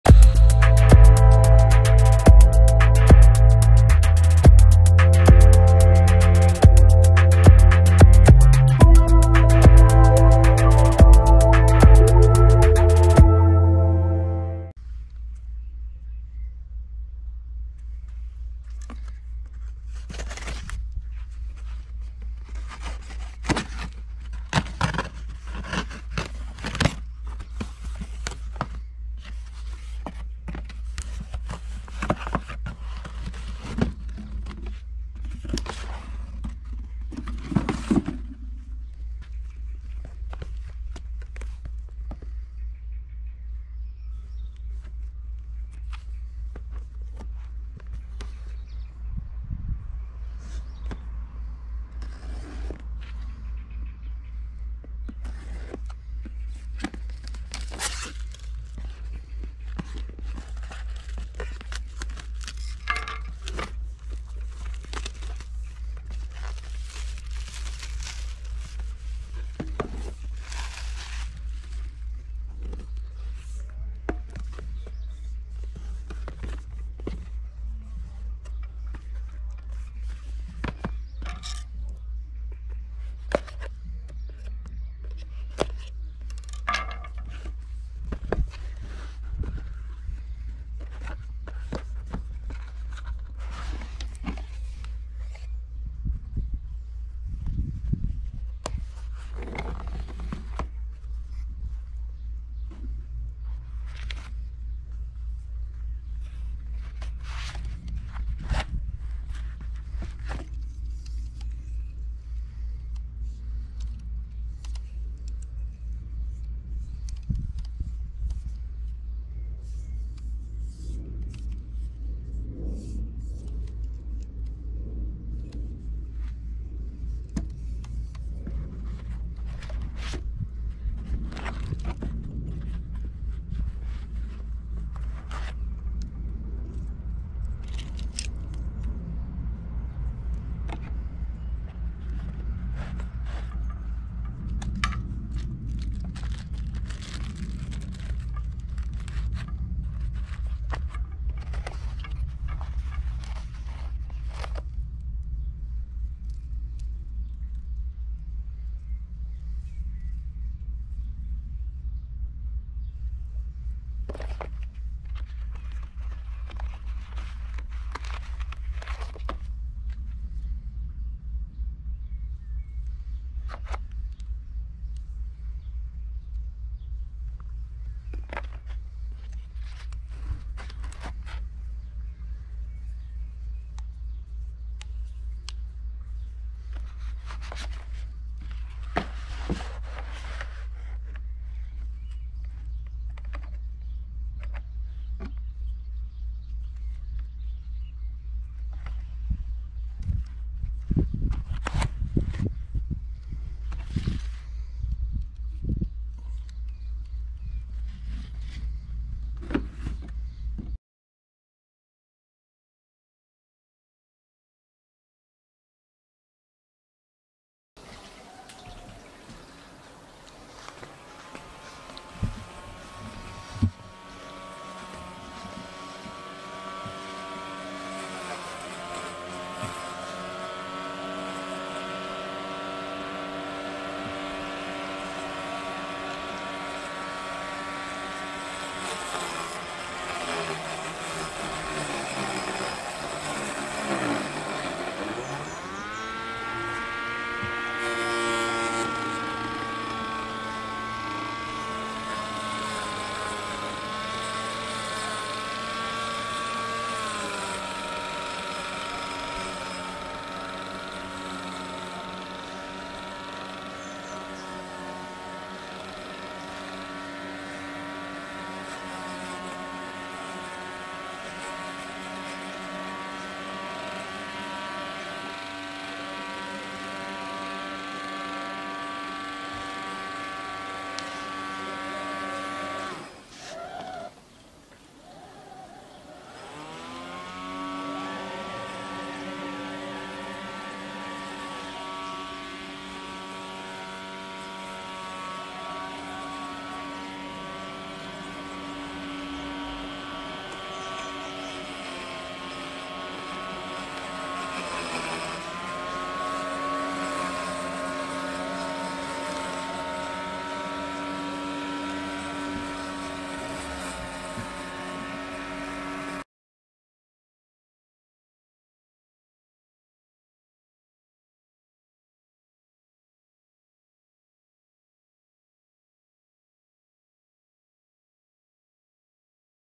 FeiyuTech AK2000C, 3 Achsen Gimbal unboxing
Das Gimbal arbeitet ganz gut.Mit der Kamera bringt es allerdings 2,5 Kg auf die Wage.Also zum Wandern ungeeignet.Ich habe das Gimbal weitreichend getestet . Es arbeitet genau so wie die anderen von mir getesteten Gimbal einwandfrei.